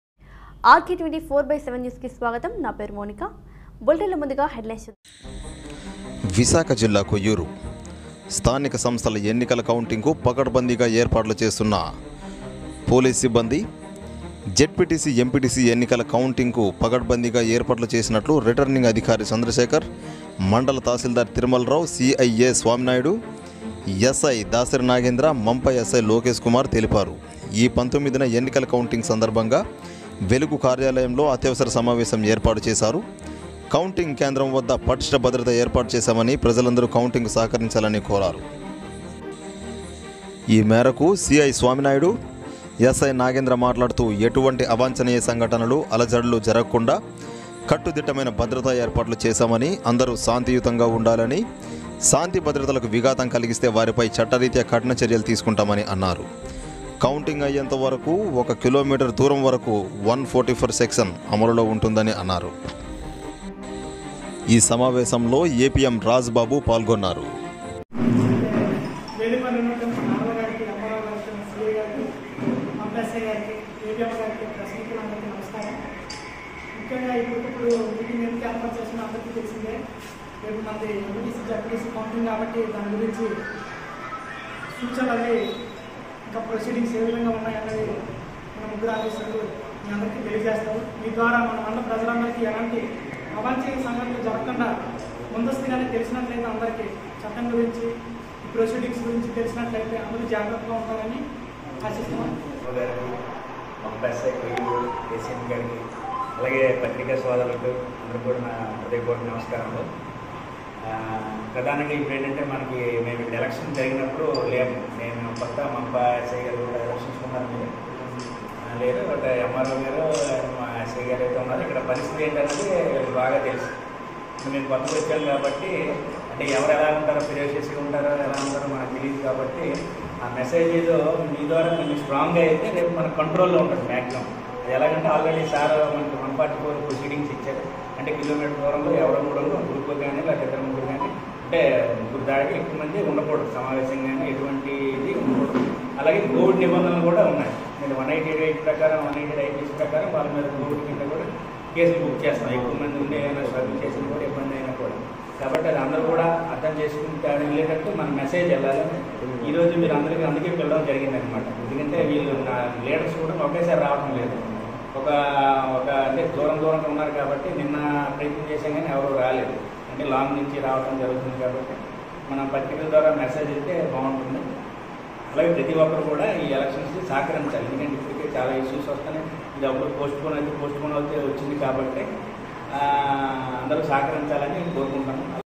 కొయ్యూరు-ZPTC,MPTC ఎన్నికల కౌంటింగ్ కు భద్రతా ఏర్పాట్లు చేసిన పోలీస్ సిబ్బంది.
విశాఖ జిల్లా, కొయ్యూరు మండలం.

ఎటువంటి అవాంచనీయ సంఘటనలు జరగకుండా భద్రతా ఏర్పాట్లు చేసిన పోలీస్ సిబ్బంది.